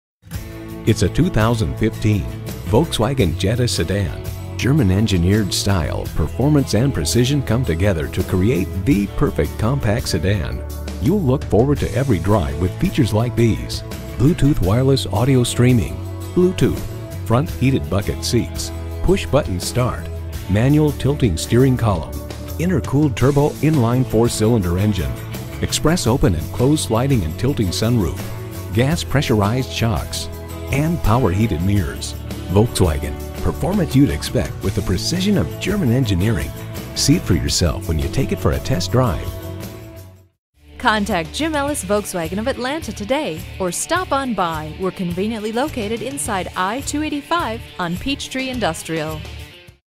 Used 2015 Volkswagen Jetta Atlanta Alpharetta, GA V2988 - SOLD
Year: 2015
Make: Volkswagen
Model: Jetta
Trim: 1.8T SE
Engine: 1.8L I4 DOHC Turbocharged
Transmission: 6-Speed Automatic with Tiptronic
Color: Black
Interior: Cornsilk Beige
Mileage: 27166
Stock #: V2988
VIN: 3VWD07AJ6FM341472
Clean CARFAX. Certified. Jetta 1.8T SE w/Connectivity CARFAX One-Owner.brbrCLEAN CAR FAX, TOUCH SCREEN RADIO, 1 OWNER, LEATHER SEATS, HEATED SEATS, SUNROOF, MOONROOF, SIRIUS XM RADIO, BLUETOOTH, Jetta 1.8T SE w/Connectivity, 4D Sedan, 1.8L I4 DOHC Turbocharged, 6-Speed Automatic with Tiptronic, FWD, Black, Cornsilk Beige w/V-Tex Leatherette Seat Trim, Power Sunroof, Rear-View Camera. 37/25 Highway/City MPG

Address:
5901 Peachtree Industry Boulevard